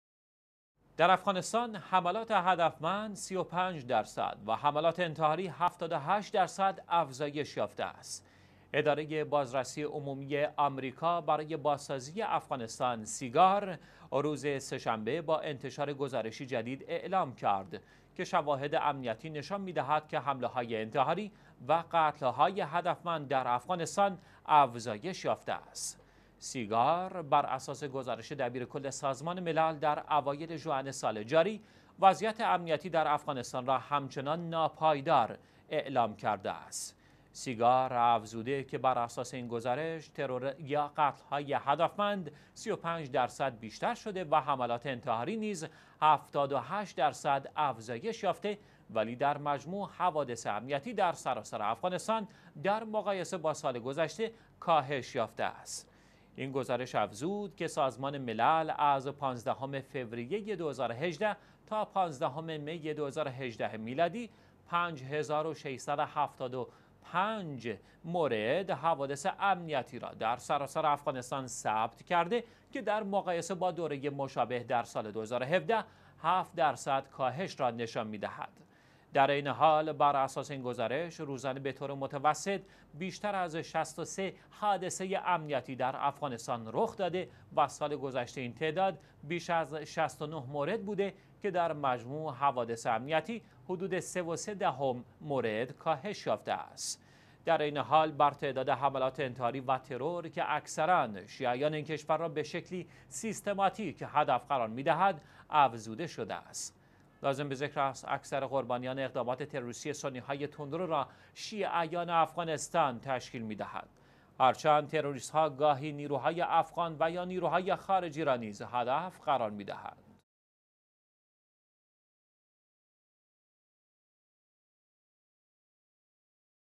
افزایش قتل‌های هدفمند و حملات انتحاری در افغانستان
حملات هدفمند ۳۵ درصد و حملات انتحاری ۷۸ درصد در افغانستان افزایش یافته است.
اداره بازرسی عمومی آمریکا برای بازسازی افغانستان (سیگار) امروز «سه شنبه» با انتشار گزارش جدیدی اعلام کرد که شواهد امنیتی نشان می‌دهد که حمله‌های انتحاری و قتل‌ های هدفمند در افغانستان افزایش یافته است.
سیگار براساس گزارش دبیر کل سازمان_ملل در اوایل ژوئن سال جاری وضعیت امنیتی درافغانستان را همچنان «ناپایدار» اعلام کرده است.
سیگار افزوده که براساس این گزارش ترورهای هدفمند و حملات انتحاری در افغانستان افزایش چشمگیر یافته است. ترور یا قتل‌های هدفمند ۳۵ درصد بیشتر شده و حملات انتحاری نیز ۷۸ درصد افزایش یافته‌ ولی در مجموع، حوادث امنیتی در سراسر افغانستان نسبت به سال گذشته کاهش یافته است.
این گزارش افزود که سازمان ملل از ۱۵ فوریه ۲۰۱۸ تا ۱۵ می ۲۰۱۸ میلادی ۵ هزار و ۶۷۵ مورد حوادث امنیتی را در سراسر افغانستان ثبت کرده که نسبت به دوره مشابه در سال ۲۰۱۷ هفت درصد کاهش را نشان می‌دهد.
در عین حال، براساس این گزارش روزانه به طور متوسط بیشتر از ۶۳ حادثه امنیتی در افغانستان رخ‌داده که سال گذشته این مقدار بیش از ۶۹ مورد بوده و حوادث امنیتی حدود ۳.۳ مورد نسبت به سال قبل کاهش یافته است. در عین حال، بر موارد حملات انتحاری و ترور که اکثرا شیعیان این کشور را به شکلی سیستماتیک هدف قرار می دهد، افزوده شده است.
لازم به ذکر است، هر چند تروریست ها گاهی نیروهای افغان و یا نیروهای خارجی را هدف قرار می دهند، امام اکثر قربانیان اقدامات تروریستی این سنی های تندرو را شیعیان افغانستان تشکیل می دهند.

چهارشنبه - ۷ شهریور ۱۳۹۷
الأربعاء - ١٧ ذوالحجه ١٤٣٩